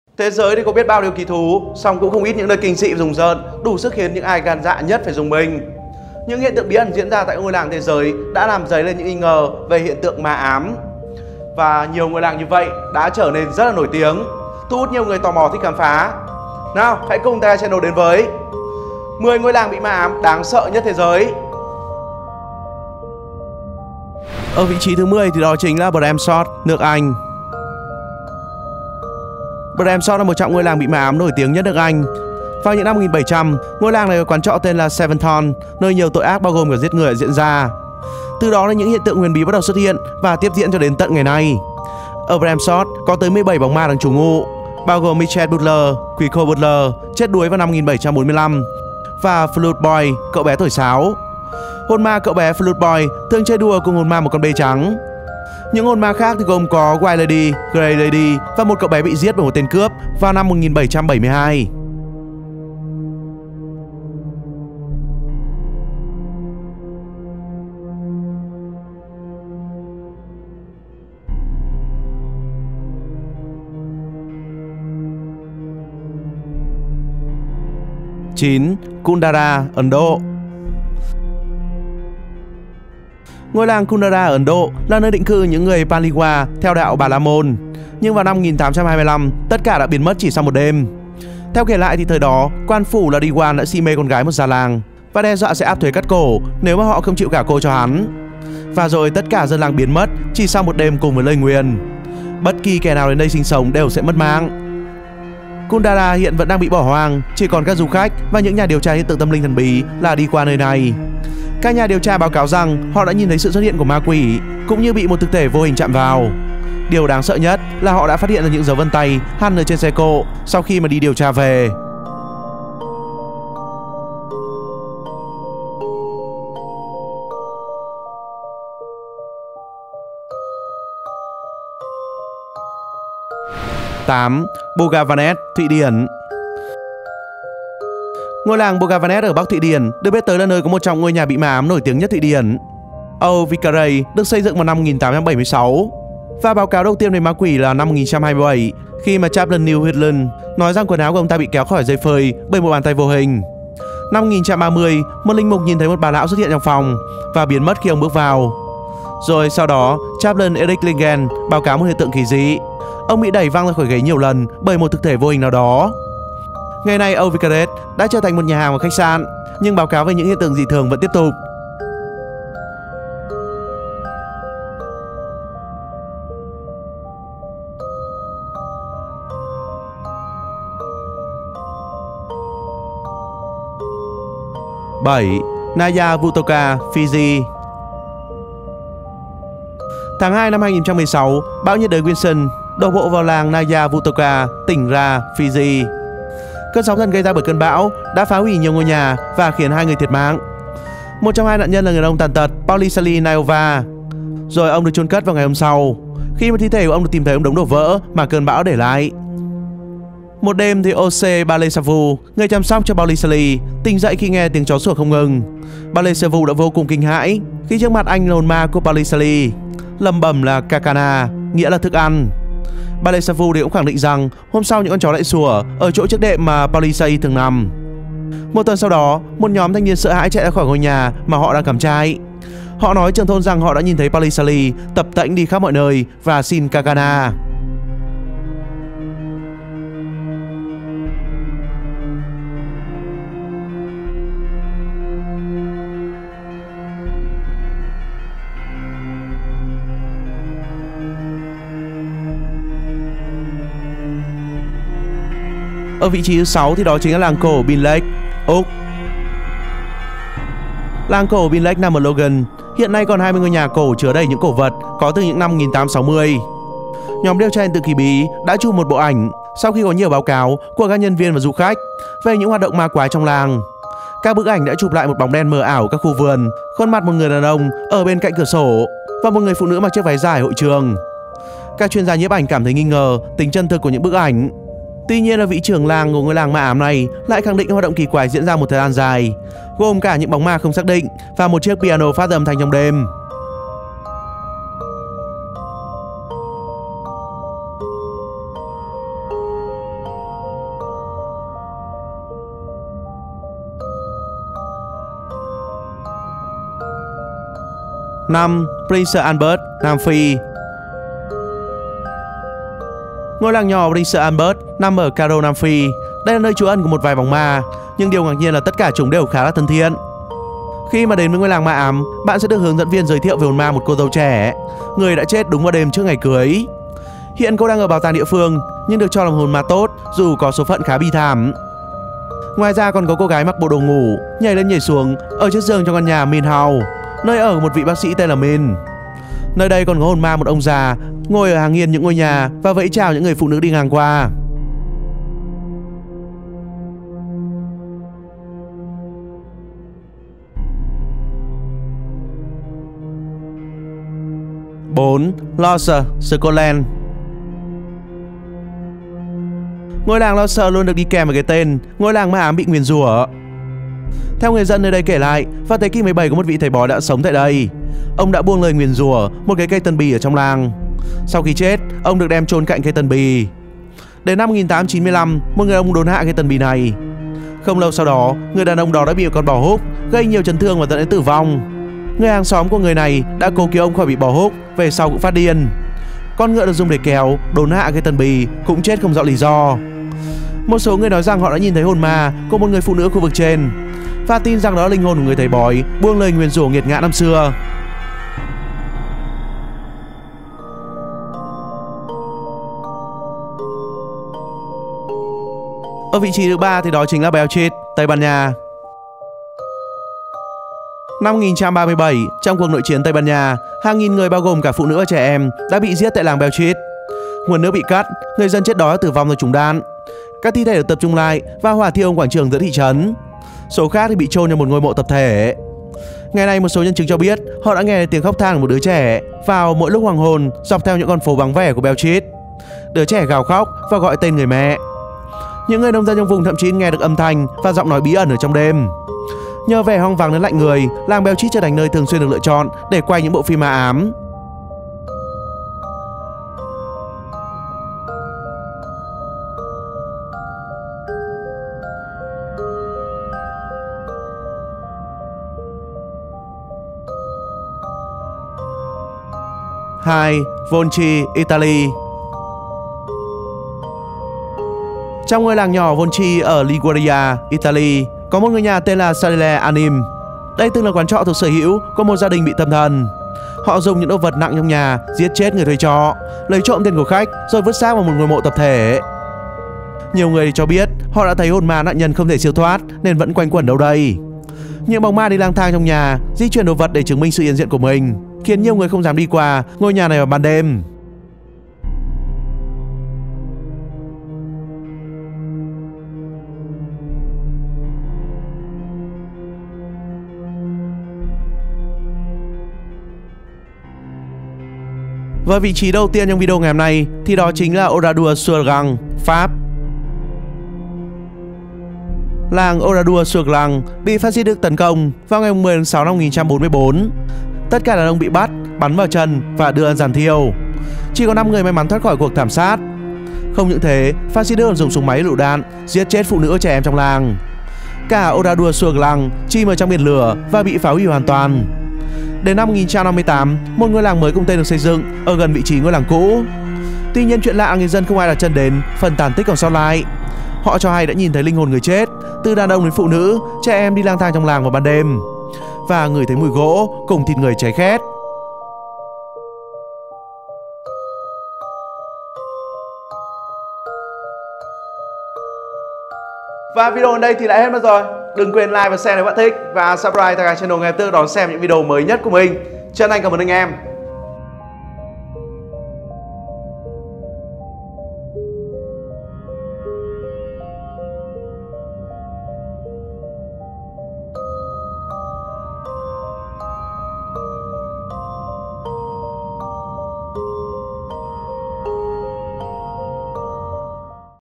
10 Ngôi Làng "QUỶ ÁM" Nơi Hồn Ma Sống Chung Với Con Người
10 Ngôi Làng "QUỶ ÁM" Nơi Hồn Ma Sống Chung Với Con Người.

Còn bạn, bạn thấy video này thế nào. Để lại ý kiến của mình trong phần bình luận nhé ^^

10 Ngôi Làng "QUỶ ÁM" Nơi Hồn Ma Sống Chung Với Con Người.